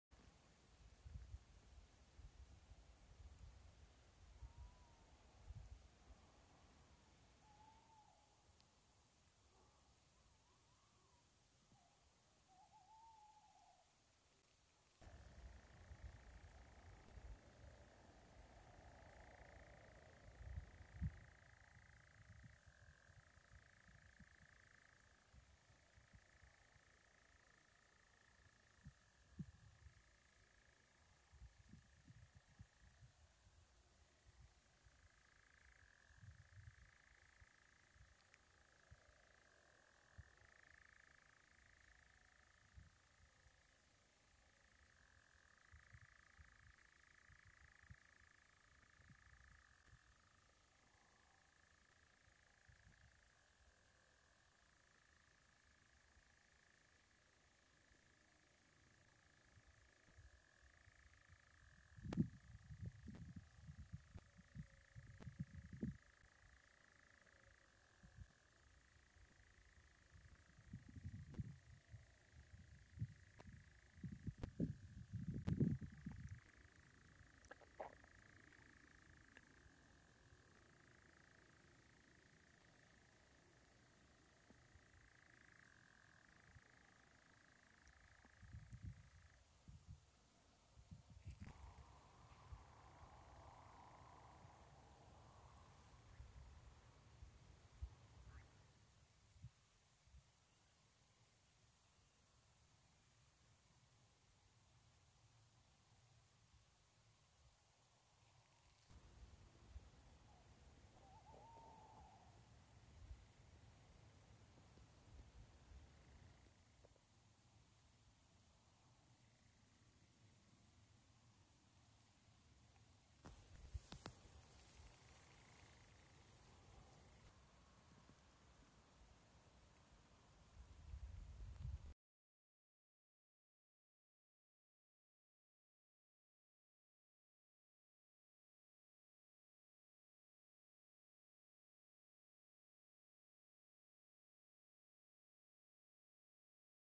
220622 Nightjars seen in Guisborough Woods
Nightjar, Tawny Owl in Guisborough Woods